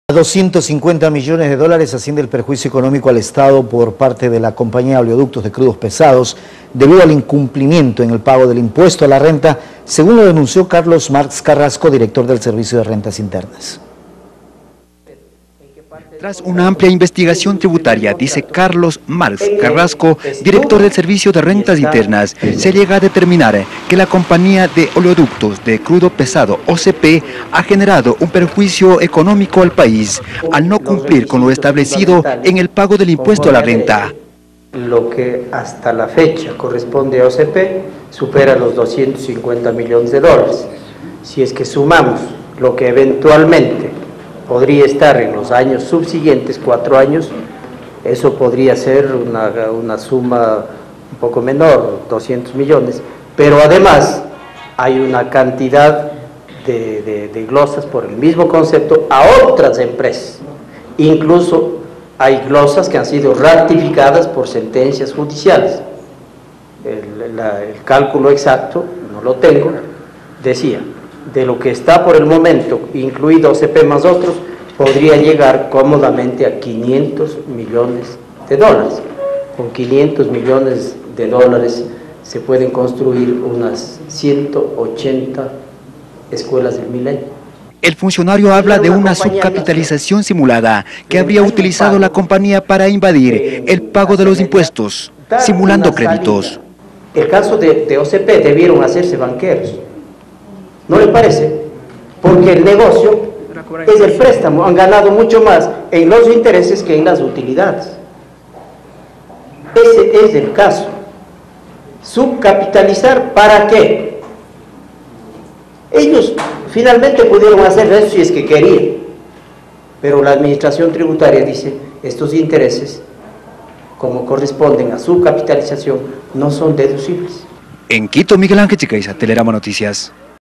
OCP habría incumplido pago de impuesto a la renta
A 250 millones de dólares asciende el perjuicio económico al estado por parte de la compañía oleoductos de crudos pesados, debido al incumplimiento del pago del impuesto a la renta, según lo denunció Carlos Marx Carrasco, director del  servicio de  rentas  internas...